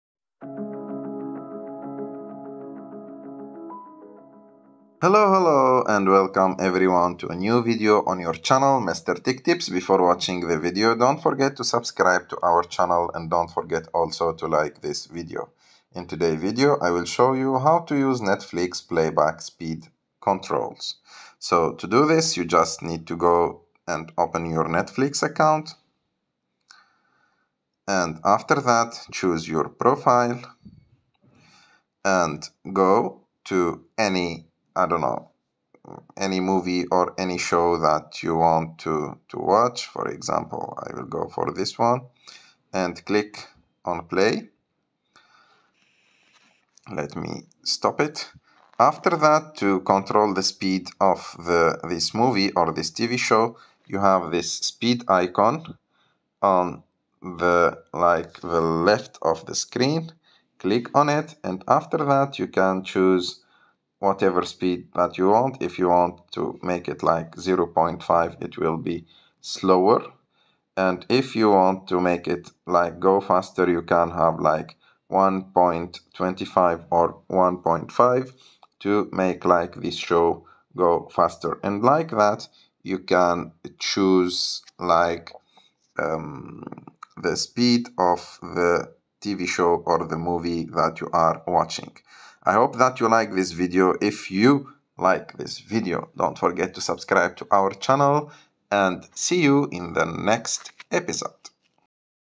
How To Change Playback Speed on Netflix (2024 Guide)
Want to watch Netflix shows and movies at your own pace? This video shows you how to use Netflix's playback speed controls to speed up, slow down, or even rewind your favorite content.